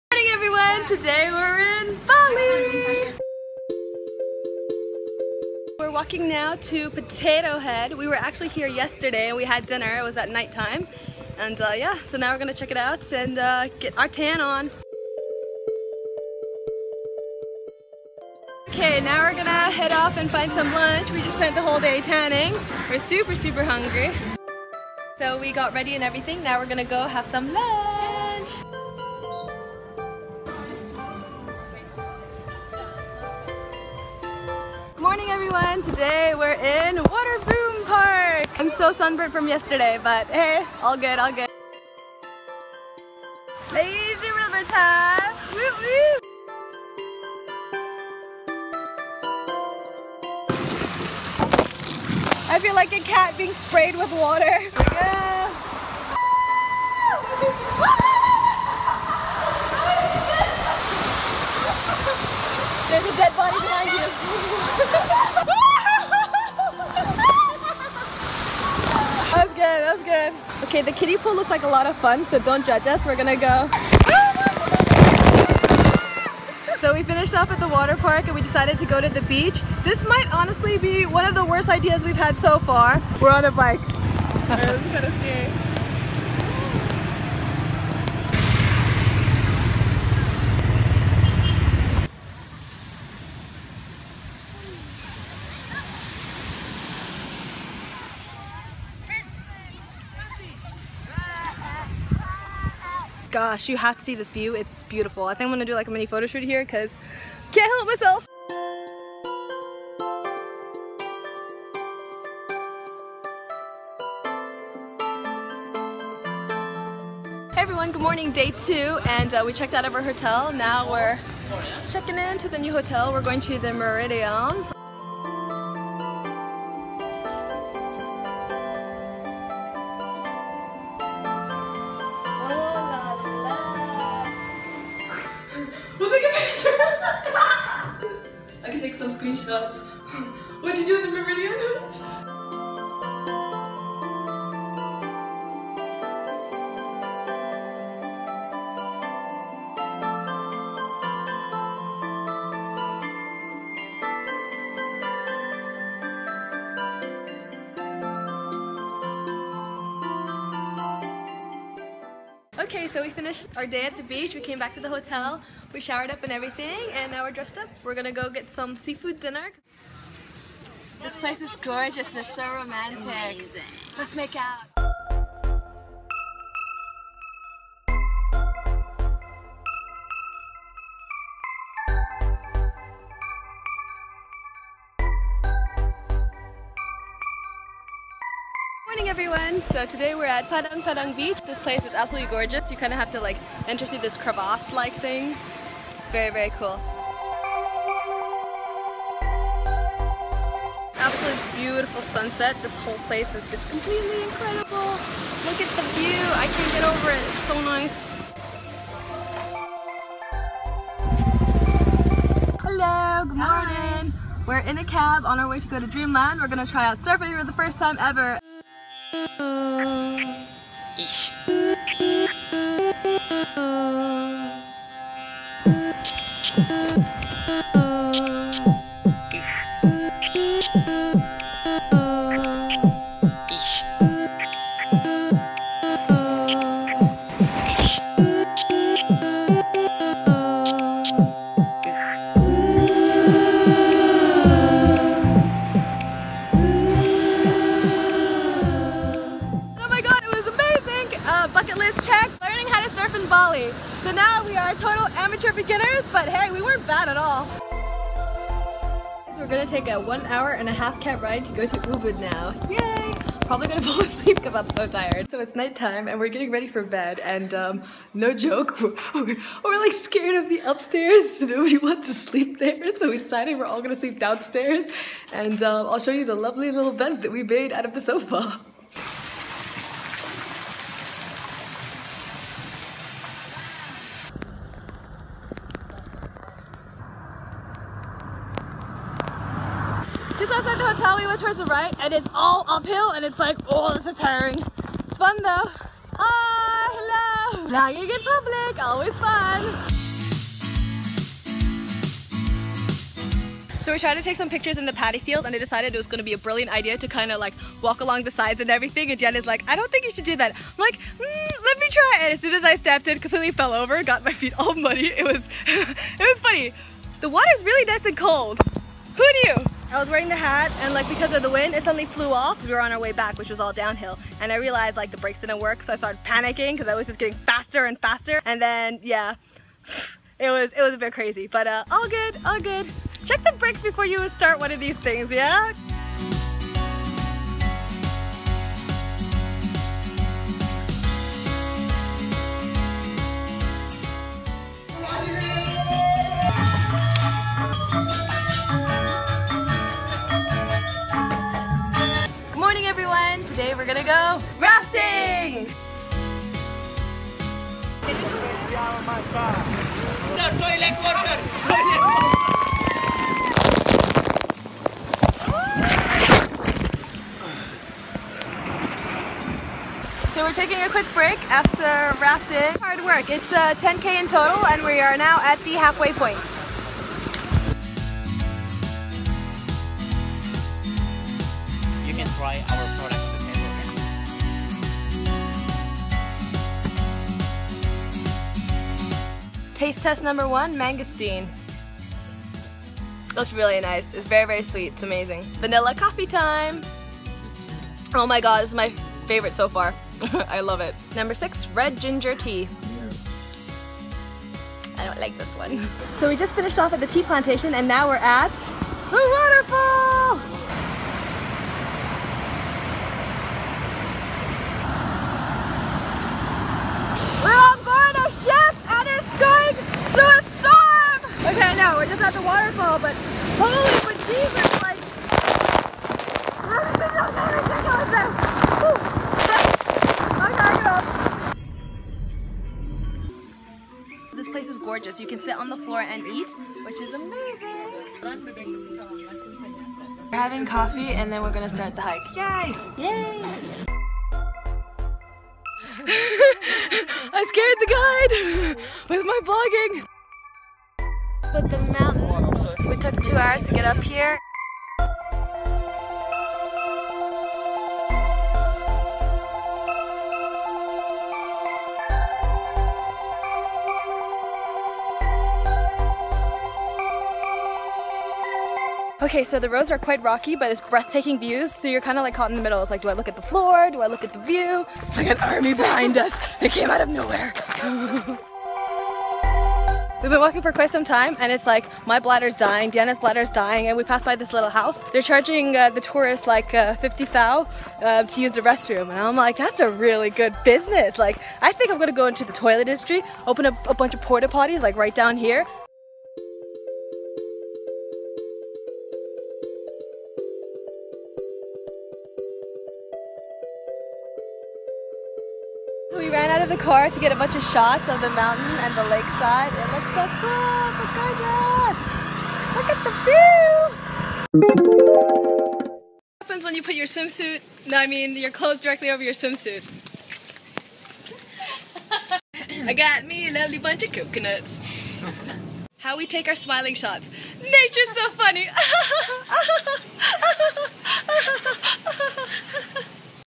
Vlog #32 Amanikins beaching in Bali!
Amanikins in beaching it up in Bali!

What can I say? Bali is amazing, did everything from surfing, white water rafting, monkey temple, ubud palace show, waterbom park and went to 4 beaches. Had a blast, would totes recommend and go back! I love asian countries, salt of the earth people, beautiful weather and nature galore!

Filming: Me
Editing: @deenapie